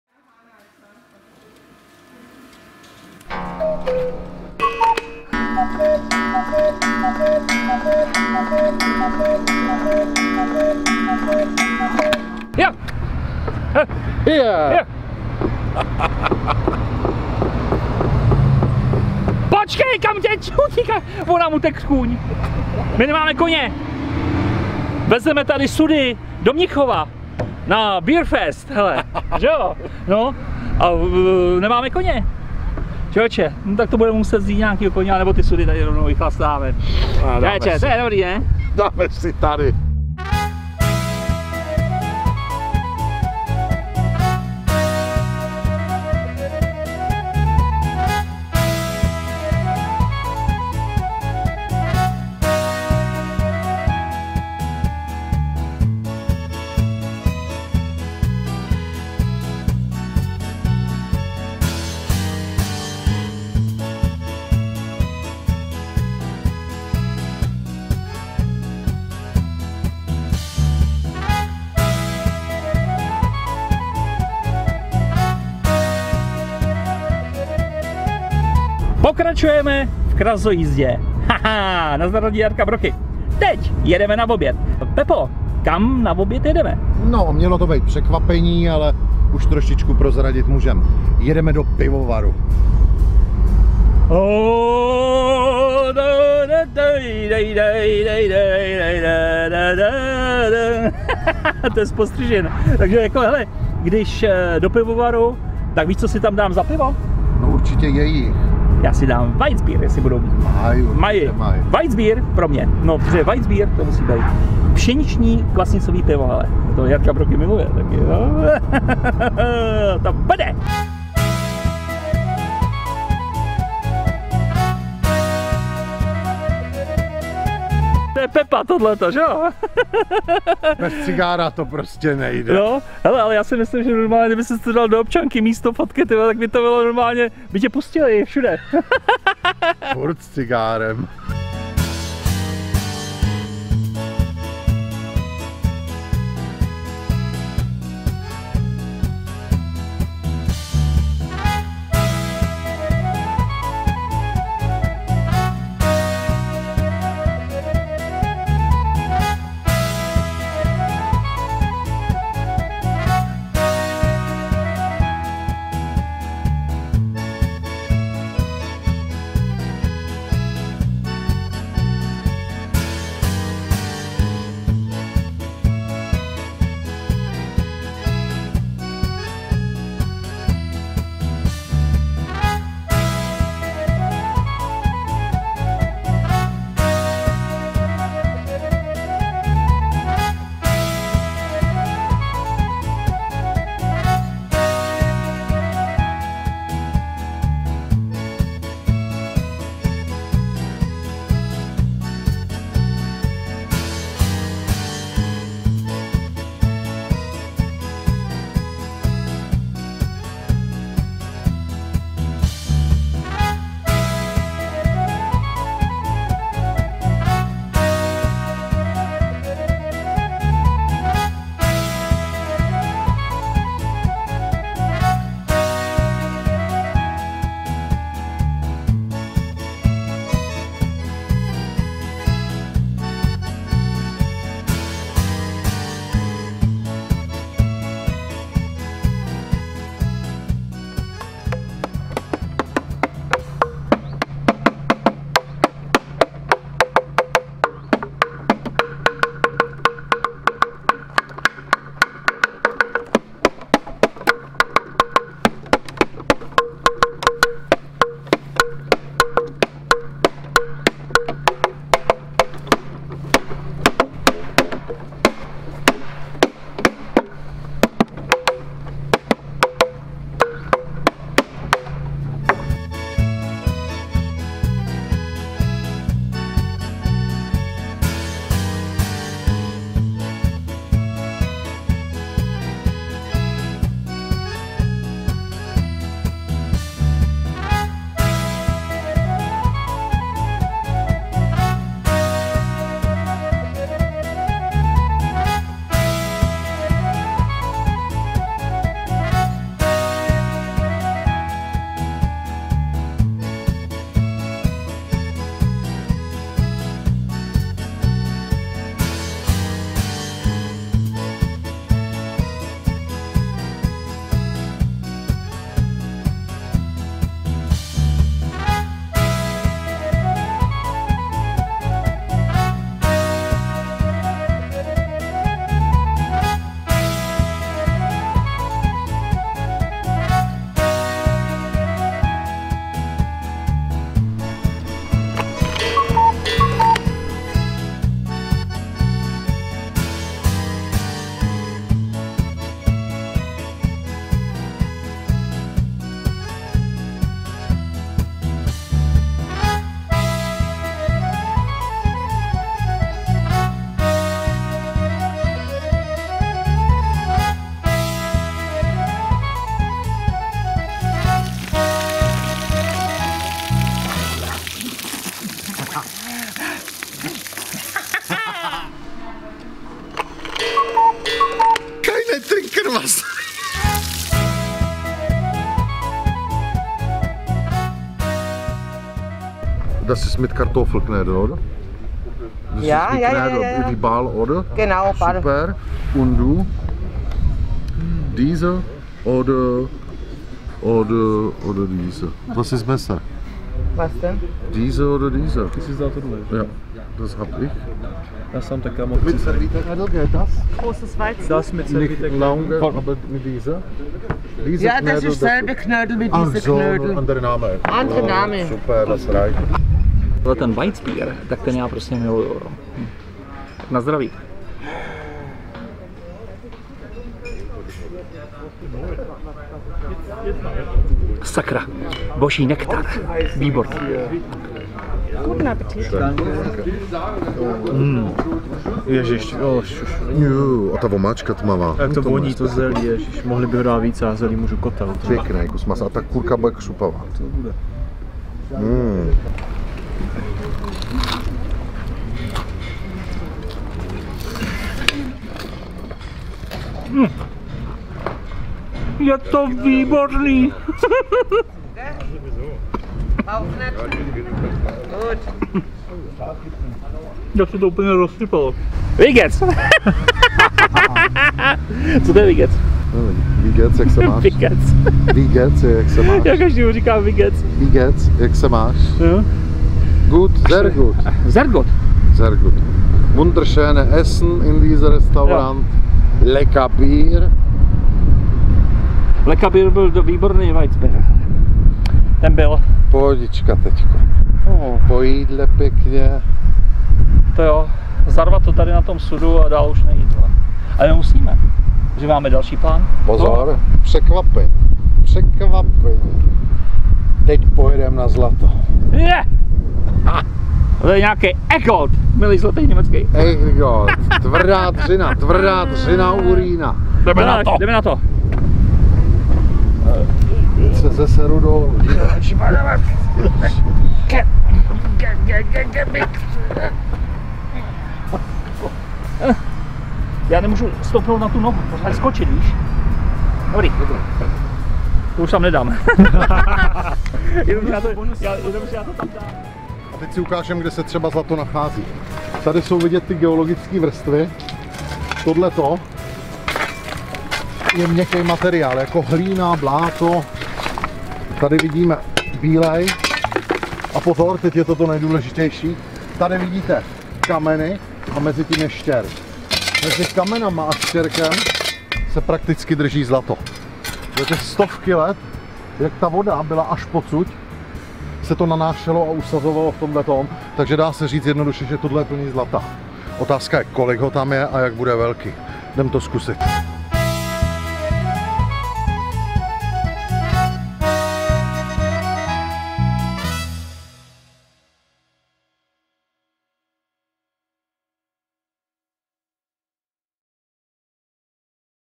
Pivovarské křupavé kolínko
Zlatonosná cesta Jarky Broki * 4.díl
I na takovém vejletíku musíte být dobře najezenééé.
Pepa to věděl a proto zvolil báječnou restauraci
v pivovaru Rothaus, posazeném na severním okraji
vesnice Grafenhausen v jižním Černém lese.
Jedná se o jeden z nejoblíběnějších a neúspěsnějších
regionálních pivovarů v Německu.
Včak také pivíčko a krmě stála za to.
Ovšem,
před vydatným obědem malá procházka
parkovým lesíkem s názvem Schlühüwanapark.
Jsou zde sochy, vytvořené z kořenů a pozůstatků
stromů hurikánu Lothar (oblast zasažena v roce 1999
amatérským řezbářem Herbertem Hofmeierem.
Velice oblíbená procházka
pro rodiny s dětmi.
A pak si už dá
výborné kolínko pečené na černém pivu.
MERCH Jarka Broki pořídíte vyhledáním na stránkách  ZDE:
...vyhledejte :
Motivy / Youtubeři / Museři / Jarka Broki
Ve střihu použita skladba :
Něco málo o Jaroslavě Brokešové alias Jarce Broki
MOTTO:
Přežít, užít, bojovat,
z ničeho se nep@srat 😀